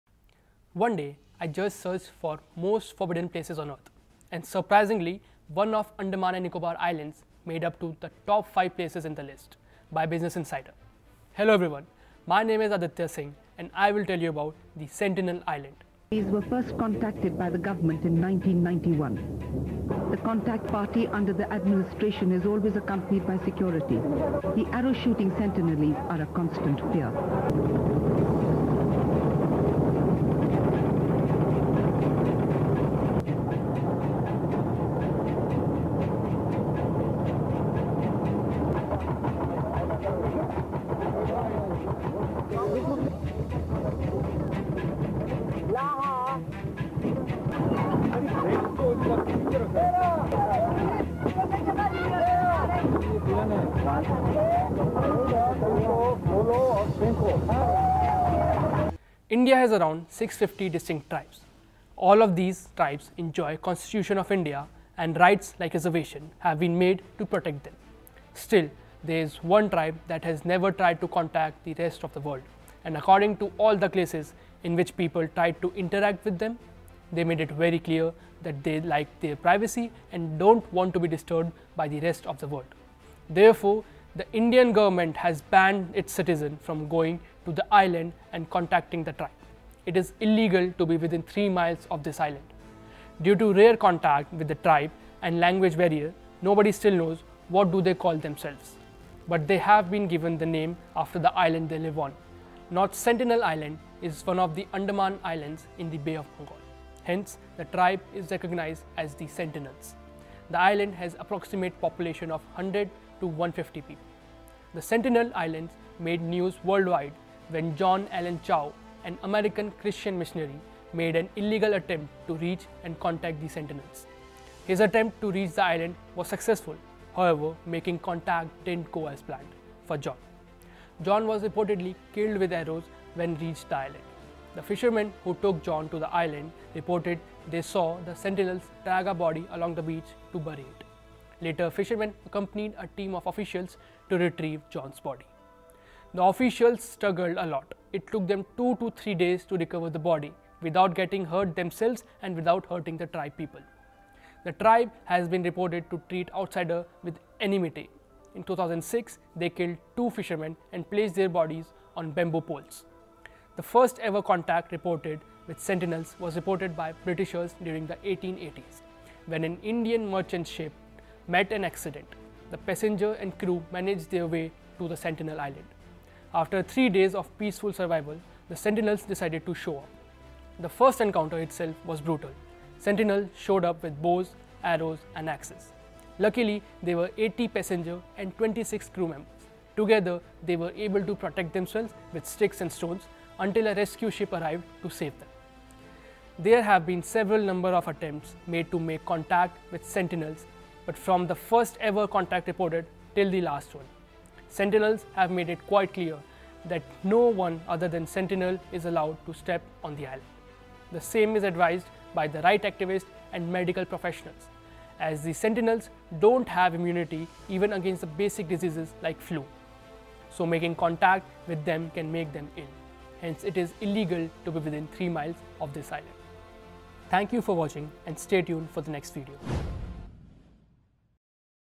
Its Illegal to go to this place in India The Sentinel Island : TheForbidden place
The sentinel island made news worldwide when John Allen Chau an American Christian missionary, made an illegal attempt to reach and make contact with the sentinels, his attempt to reach the island was successful, however, making contact didn't go as planned for John.
Due to rare contact with the tribe and language barriers nobody still knows, what do they call themselves? But they have been given the name after the island they live on. North Sentinel Island is one of the Andaman Islands in the Bay of Bengal, hence, the tribe is recognized as "the sentinels."

Sound Effect by a href=httpspixabay.comuserscristian_changing-30278997utm_source=link-attribution&utm_medium=referral&utm_campaign=music&utm_content=121425Cristian_Changinga from a href=httpspixabay.comutm_source=link-att